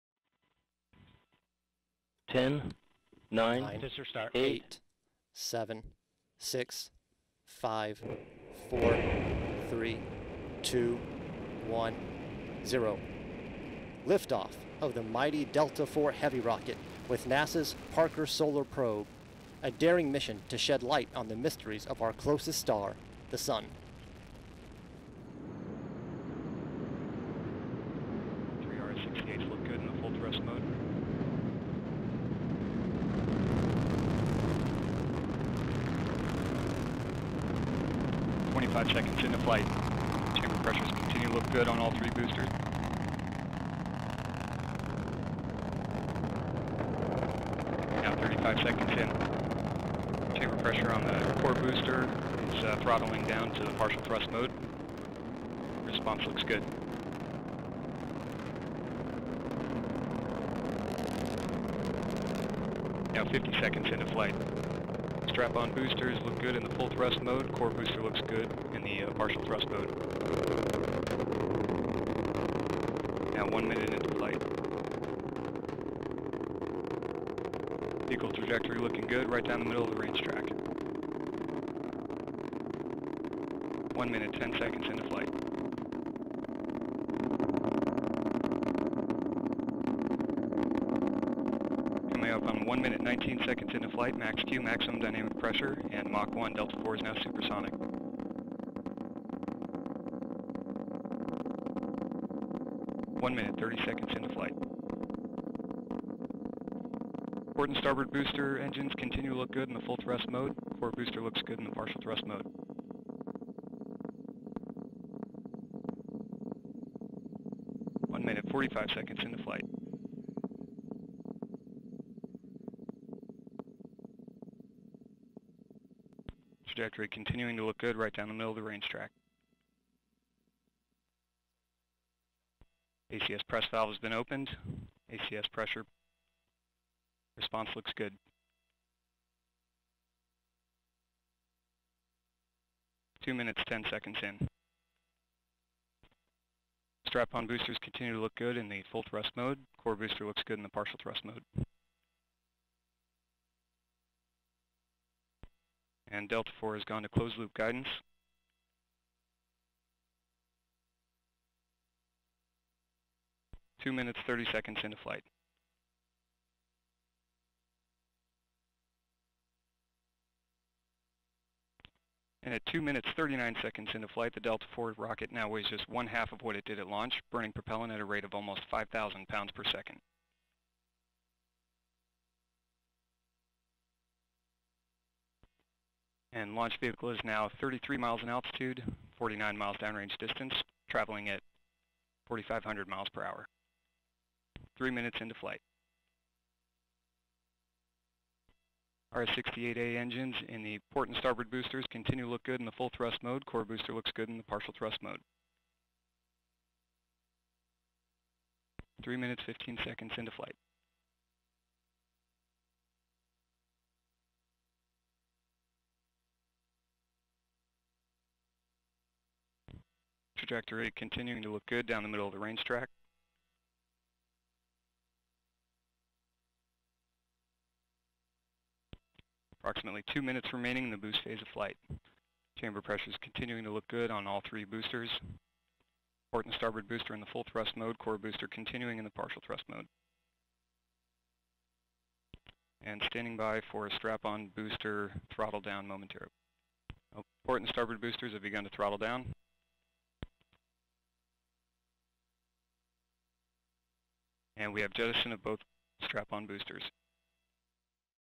Liftoff of NASA's Parker Solar Probe
A United Launch Alliance Delta IV Heavy rocket lifts off at 3:31 a.m. EDT from Space Launch Complex 37 at Cape Canaveral Air Force Station carrying NASA's Parker Solar Probe on a mission to the Sun. The mission will perform the closest-ever observations of a star when it travels through the Sun's atmosphere, called the corona. The probe will rely on measurements and imaging to revolutionize our understanding of the corona and the Sun-Earth connection.